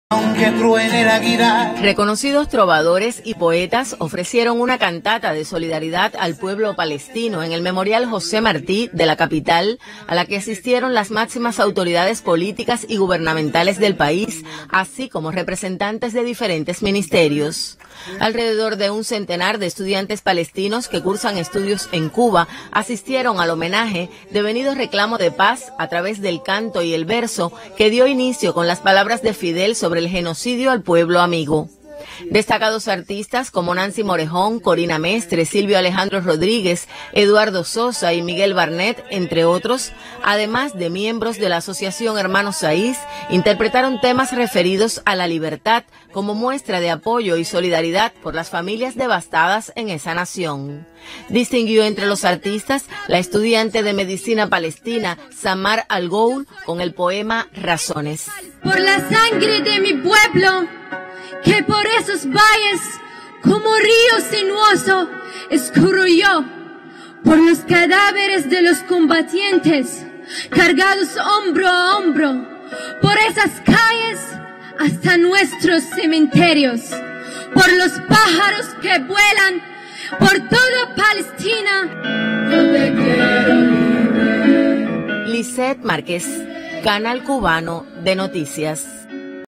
Cuba/La Habana: Realizan cantata de solidaridad por una Palestina Libre
“Por una PalestinaLibre” fue la cantata de Solidaridad ofrecida en el memorial José Martí, este viernes. Presidieron el homenaje al pueblo palestino el primer secretario del Comité Central del Partido, y Presidente de la República de Cuba Miguel Díaz-Canel Bermúdez, los miembros del Buró Político, Manuel Marrero Cruz, Primer Ministro, y Roberto Morales Ojeda, secretario de Organización del Comité Central del Partido, así como el embajador de Palestina en Cuba, Akram Samhan.

Hoy a las 7:00 pm se transmitirá la Mesa Redonda: Situación de Palestina